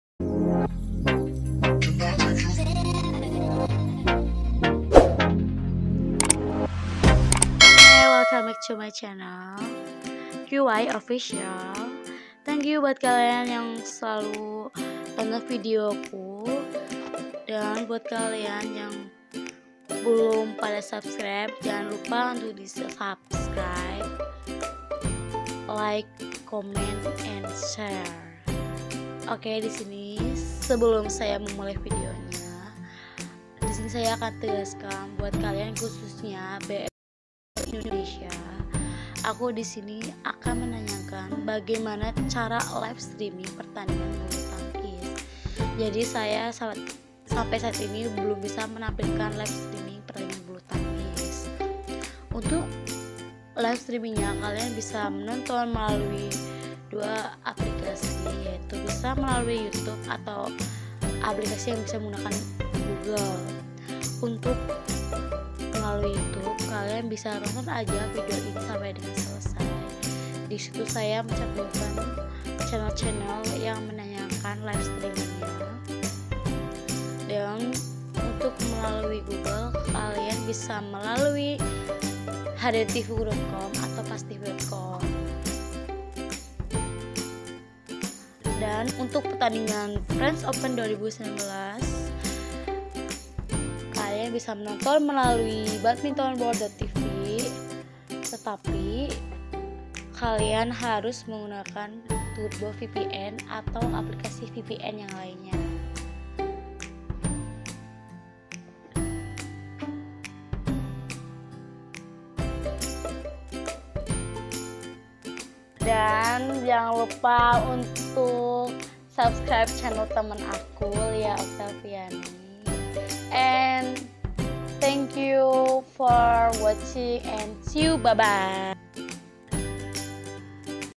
Cara Live Streaming France Open 2019 Anthony Sinisuka Ginting vs Kento Momota
Isi video tersebut menampilkan bagaimana cara Live Streaming | Live Score Badminton France Open 2019, bukan menayangkan live streamingnya ya sobat BL
Judulnya kurang sesuai karena keterbatasan huruf yg dicantumkan yah
Pake VPN Inggris
Tanpa VPN melalui channel DianAnita,com/ Bulutangkis7/ Dian Sport/Oziltv/Dian Anita Studio Live/Badminton News/KGFC Plus
follow igs @qy.official_